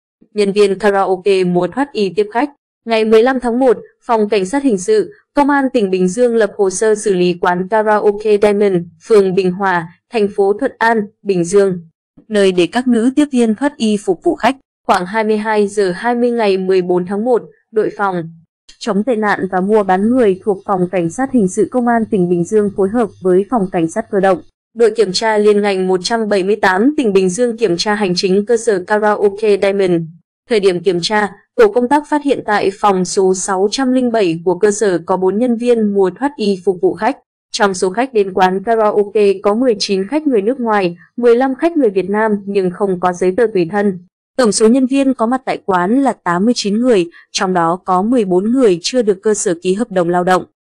Nhân viên karaoke múa thoát y tiếp khách
Ngày 15/1, Phòng CSHS, Công an tỉnh Bình Dương lập hồ sơ xử lý quán karaoke Diamond (phường Bình Hòa, TP Thuận An, Bình Dương), nơi để các nữ tiếp viên thoát y phục vụ khách.Tiếp viên quán karaoke Diamond.
Khoảng 22h 20 ngày 14/1, Đội phòng, chống tệ nạn và mua bán người thuộc Phòng CSHS Công an tỉnh Bình Dương phối hợp với Phòng Cảnh sát cơ động, Đội kiểm tra liên ngành 178 tỉnh Bình Dương kiểm tra hành chính cơ sở karaoke Diamond.
Tổ công tác kiểm tra phòng karaoke.
Thời điểm kiểm tra, tổ công tác phát hiện tại phòng số 607 của cơ sở có 4 nhân viên múa thoát y phục vụ khách. Trong số khách đến quán karaoke có 19 khách người nước ngoài, 15 khách người Việt Nam nhưng không có giấy tờ tùy thân. Tổng số nhân viên có mặt tại quán là 89 người, trong đó có 14 người chưa được cơ sở ký hợp đồng lao động.